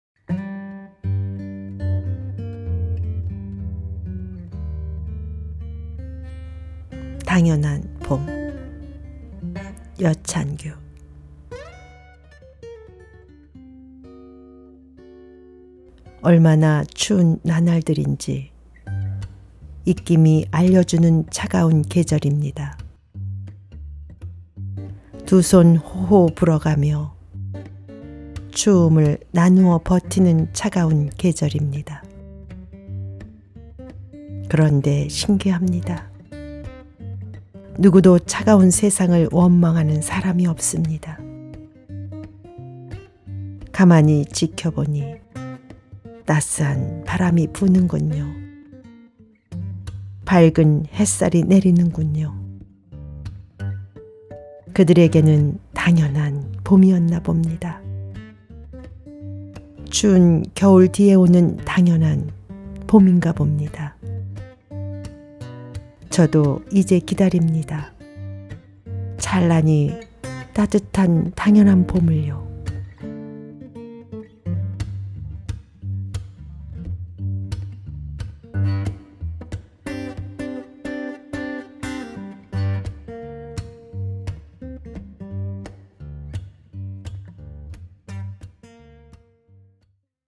[시낭송] 당연한 봄｜여찬규｜잠잘때 듣는｜힐링｜힐링영상｜시 읽어주는 여자｜승현 유미숙｜ASMR｜감성소풍
당연한 봄

                      시 · 여찬규
                  낭송 · 승현 유미숙

얼마나 추운 나날들인지
입김이 알려주는
차가운 계절입니다

두 손 호호 불어가며
추움을 나누어 버티는

그런데 신기합니다
누구도 차가운 세상을
원망하는 사람이 없습니다

가만히 지켜보니
따스한 바람이 부는군요
밝은 햇살이 내리는군요

그들에게는 당연한 봄이었나 봅니다
추운 겨울 뒤에 오는
당연한 봄인가 봅니다

저도 이제 기다립니다
찬란히 따뜻한
당연한 봄을요